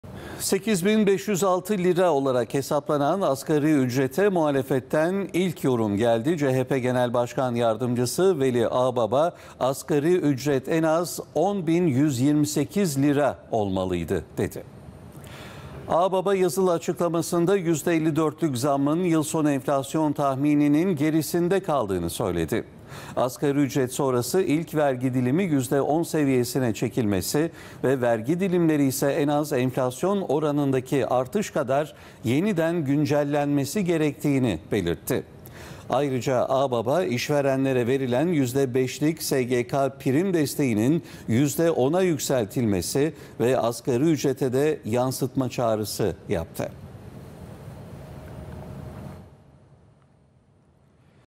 CHP'den asgari ücret yorumu: "Asgari ücret en az 10 bin 128 TL olmalıydı"
8 bin 506 TL olarak hesaplanan asgari ücrete muhalefetten ilk yorum geldi. CHP Genel Başkan Yardımcısı Veli Ağbaba, "Asgari ücret en az 10 bin 128 TL olmalıydı" dedi.